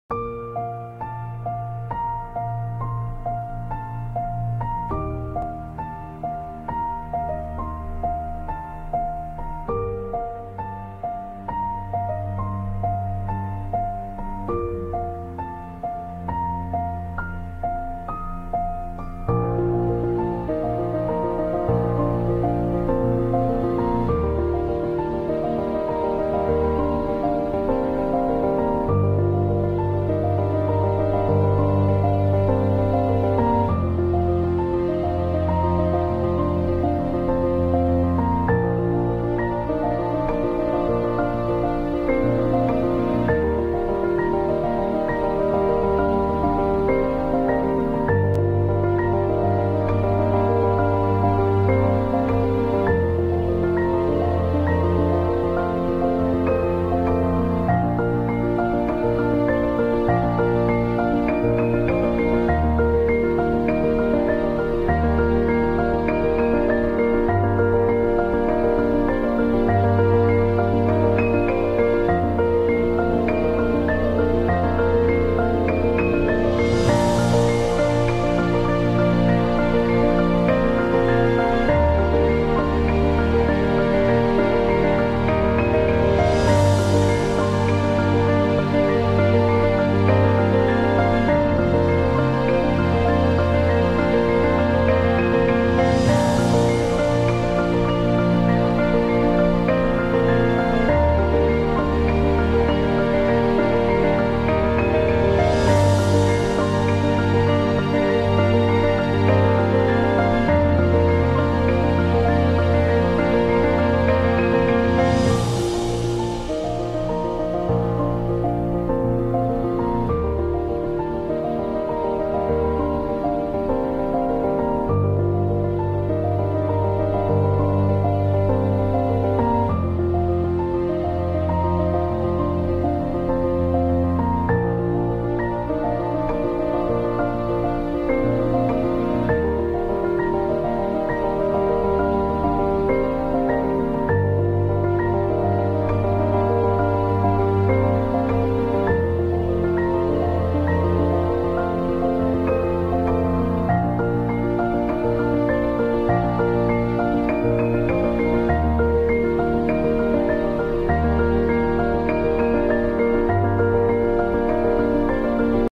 creative landscape painting
making another landscape painting in a creative way